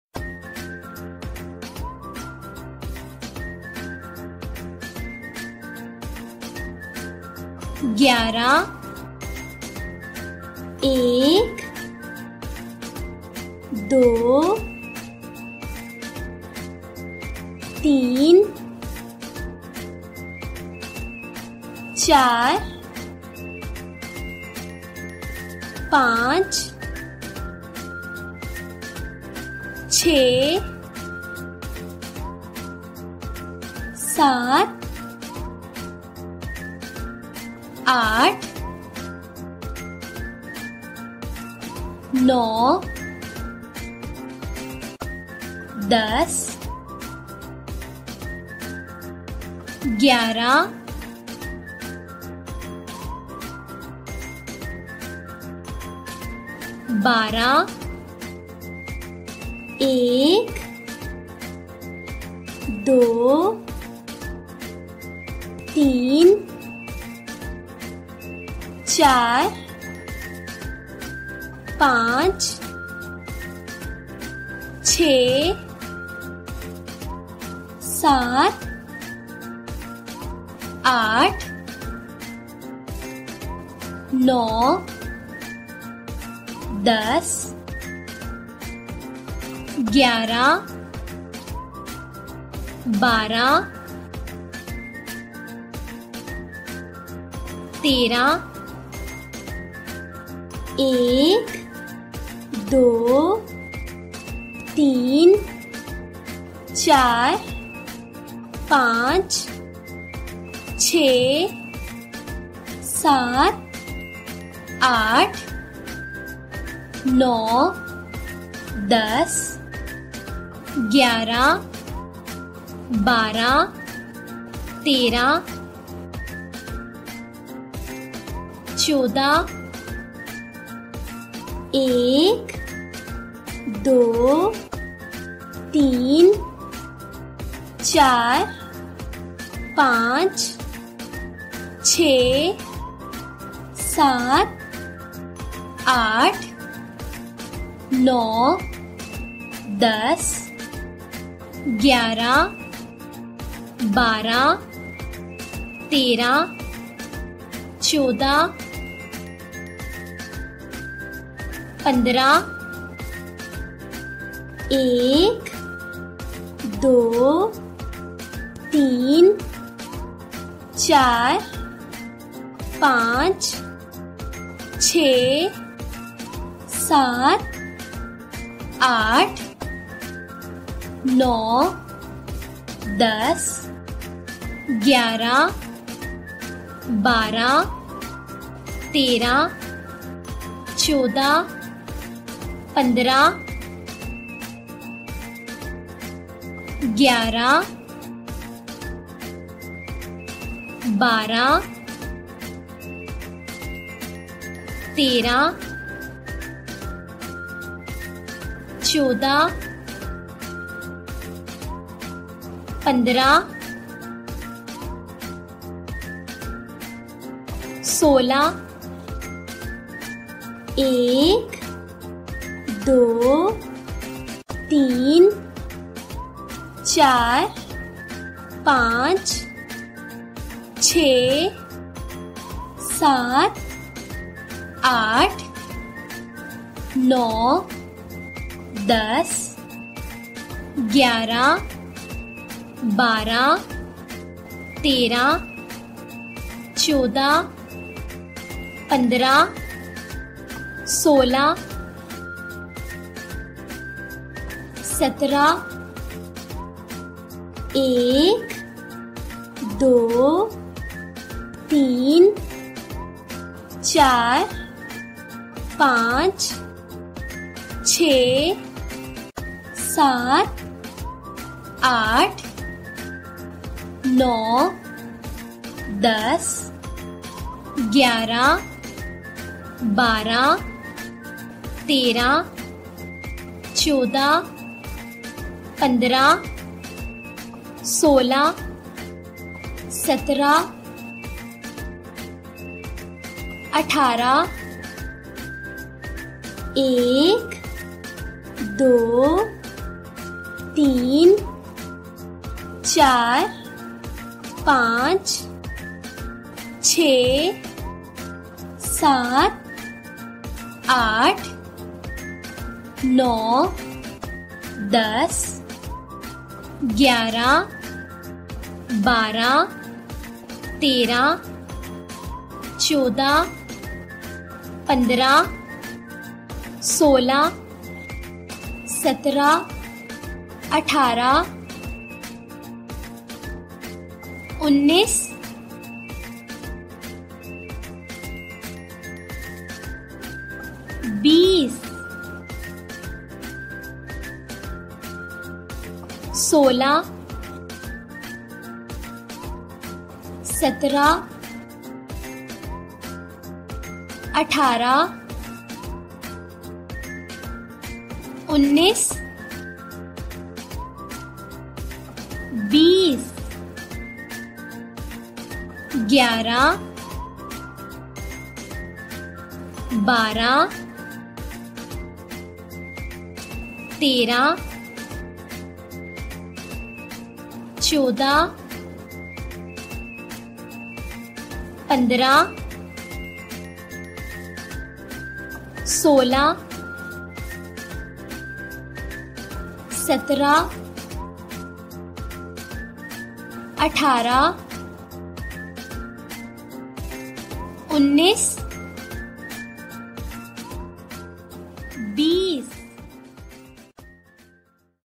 Count 11 - 20 for Kids in Urdu Easy
In this Video, Students will learn the counting from 11 to 20 in Very easy way.